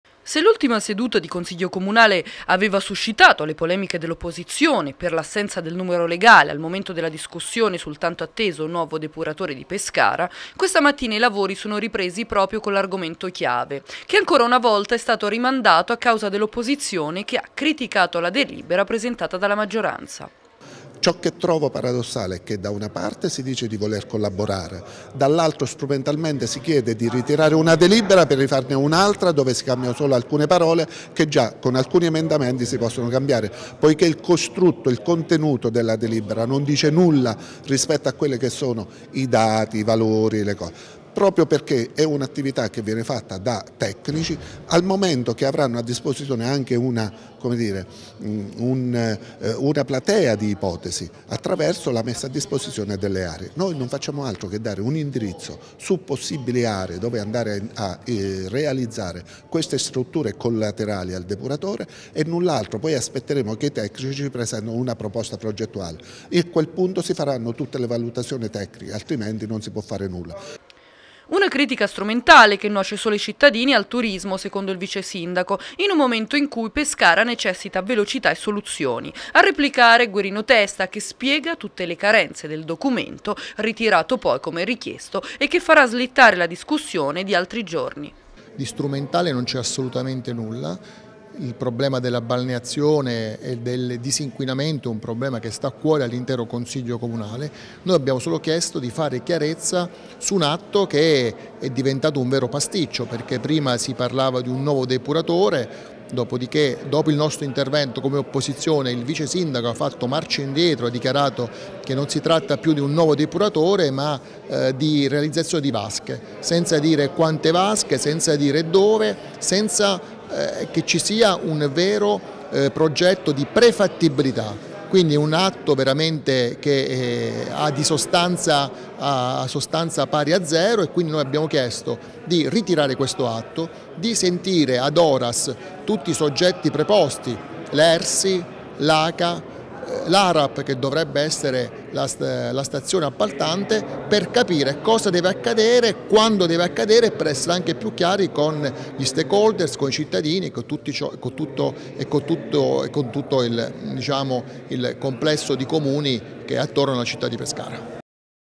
Pescara. Depuratore, nuovo rinvio in consiglio
Ancora rinviata la discussione sui lavori per il depuratore di Pescara, a far slittare la seduta di consiglio comunale questa mattina, una delibera contestata dall'opposizione.